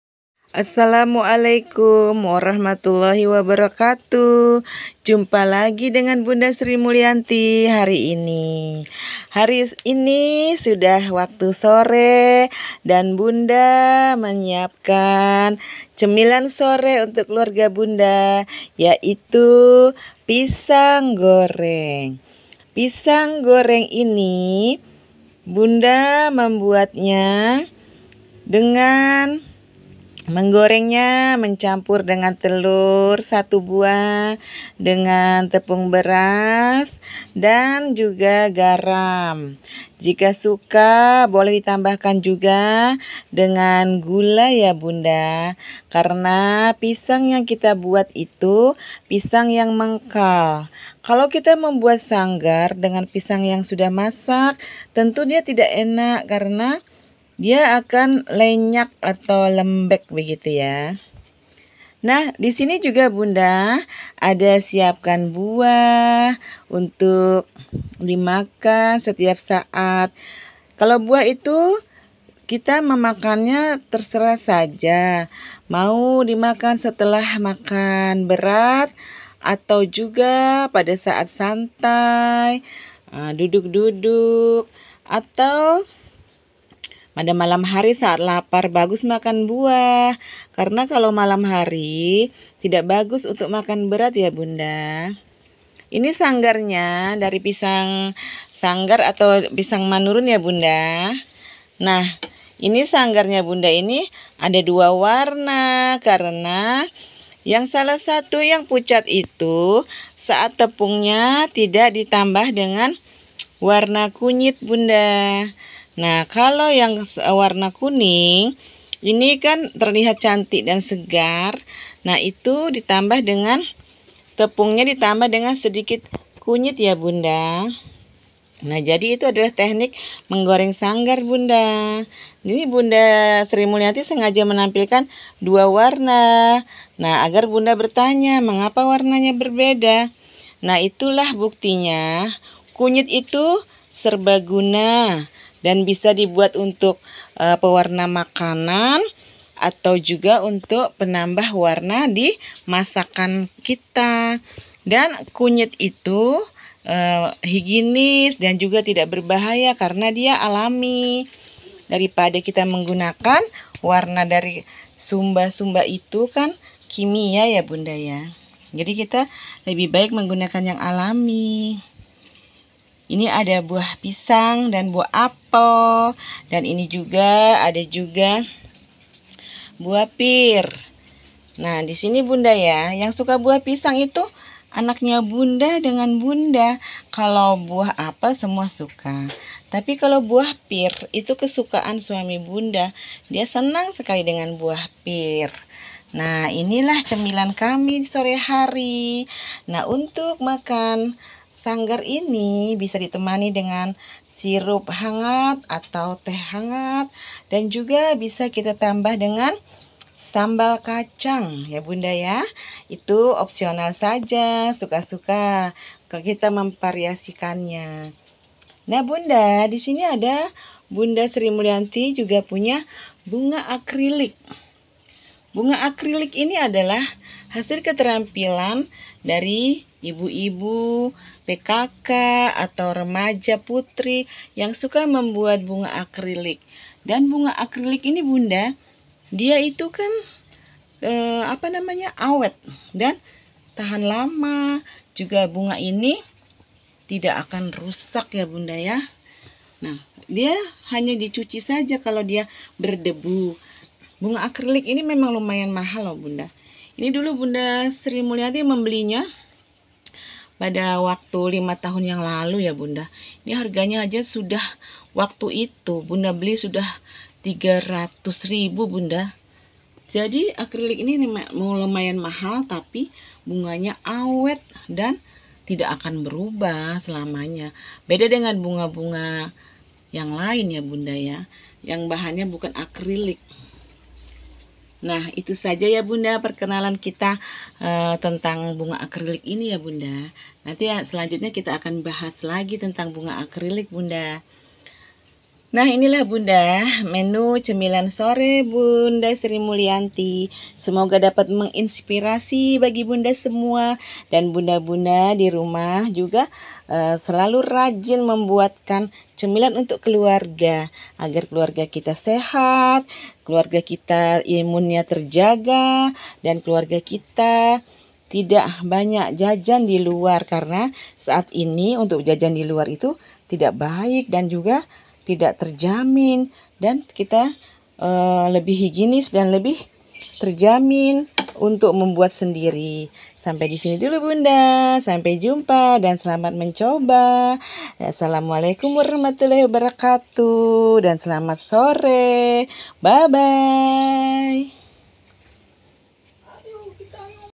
SANGGAR ATW PISANG GORENG ALA BUNDA....BUAT KUDAPAN SORE HARI .....
BAHANX pisang sanggar yg mengkal,tepung serba guna Sasa boleh ditambah tepung beras 3 sendok makan dn telur 1butir, garam secukupx ditambah kunyit halus sedikit ,boleh juga tidak ditambah kunyit halus itu opsional saja